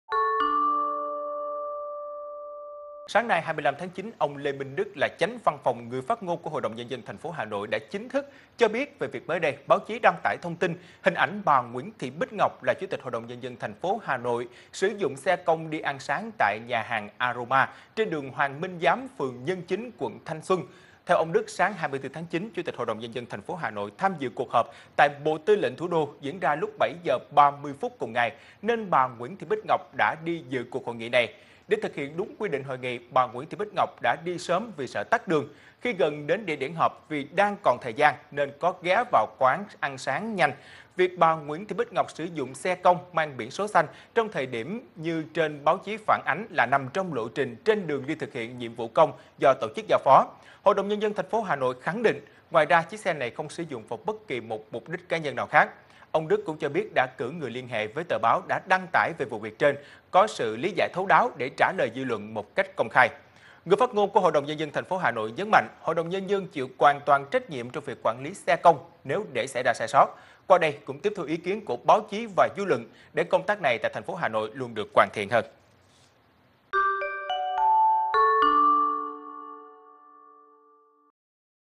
Lãnh đạo Hà Nội lên tiếng vụ dùng xe công đi ăn sáng
VTC1 | Trước thông tin Chủ tịch HĐND Hà Nội Nguyễn Thị Bích Ngọc đi xe công ăn sáng, ông Lê Minh Đức, người phát ngôn của HĐND Hà Nội đã lên tiếng xác minh thông tin vụ việc.